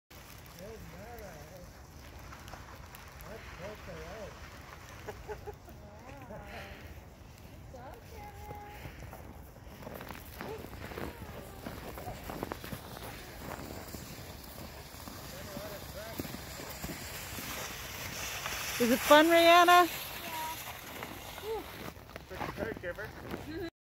St. Bernard learning to pull a sled.
Carebear is 3.5 years old and loves the snow,  kids, and playing. We thought we'd see if he likes pulling a sled. He had a blast! We'll be looking into getting him a proper harness now.